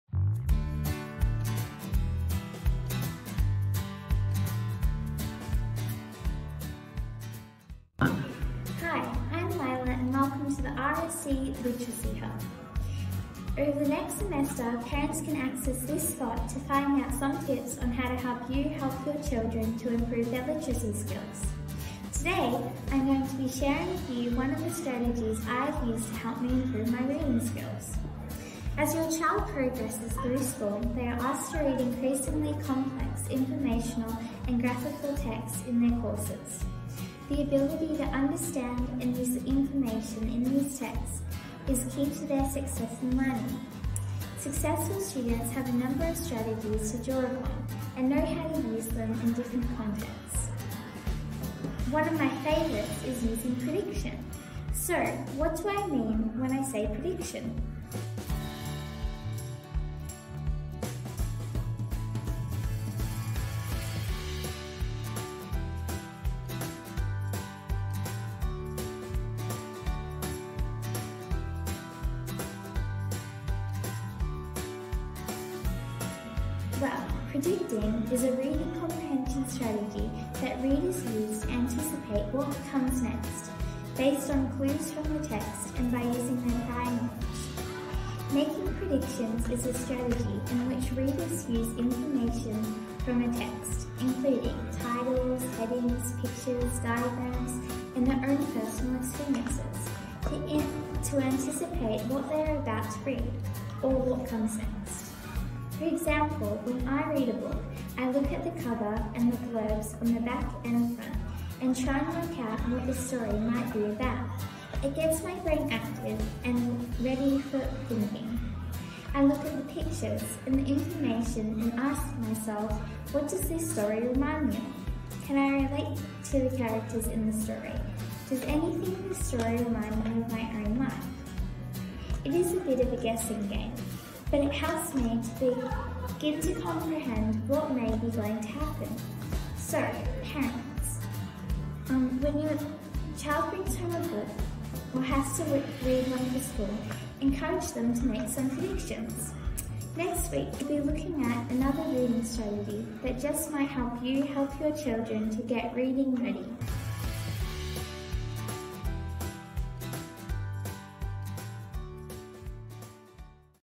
Literacy Strategy #2: Prediction
Our RSC student, Lila, shares in our second Literacy Hub video 'making a prediction strategy' and how this helps you understand the text you are reading.